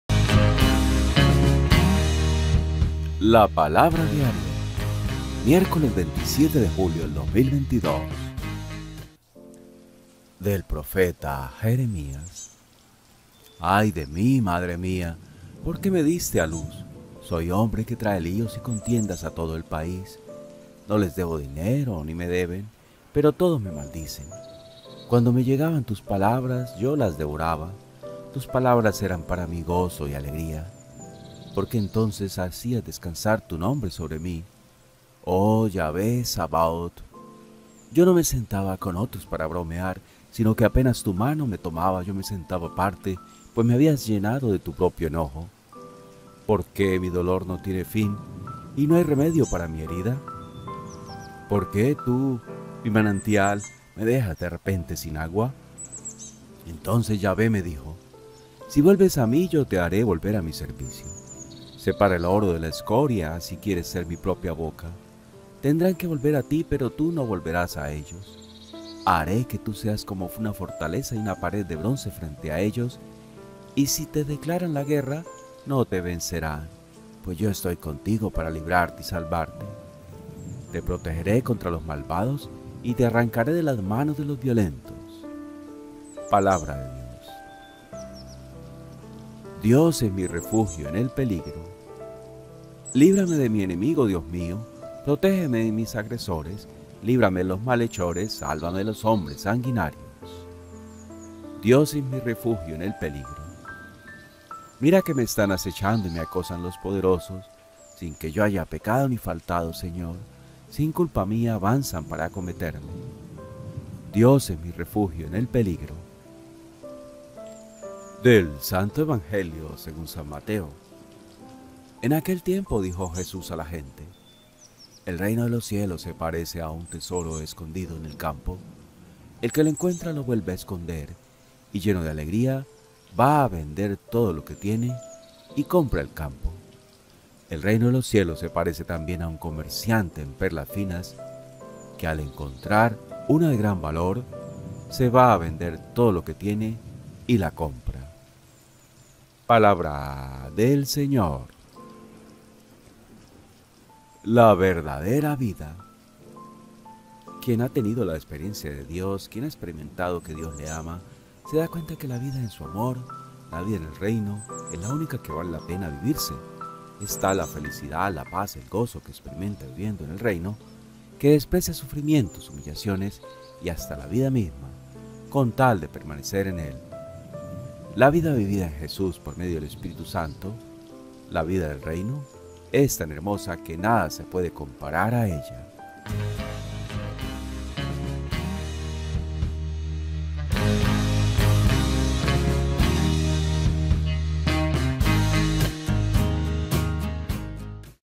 La Palabra Diaria Miércoles 27 de julio de 2022 La verdadera vida ✝️
"El Reino de los Cielos se parece también a un comerciante en perlas finas, que, al encontrar una de gran valor, se va a vender todo lo que tiene y la compra."